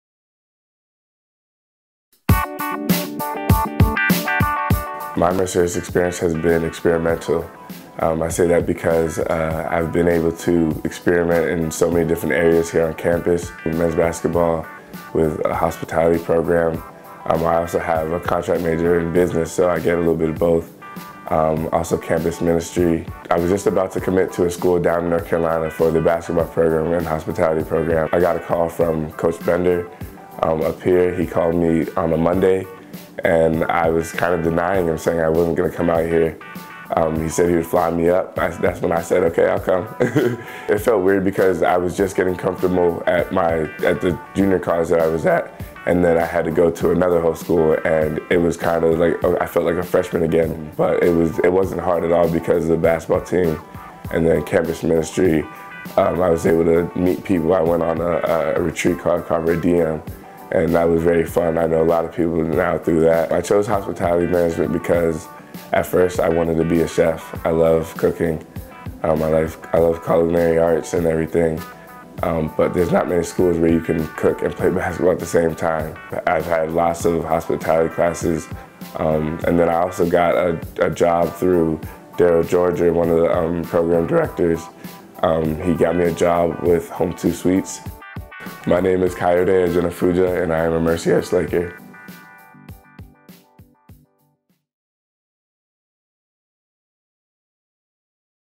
myMUexperience - Kayode Ajenifuja
Kayode is a transfer student who came to Mercyhurst for the Hospitality Management program and to play basketball.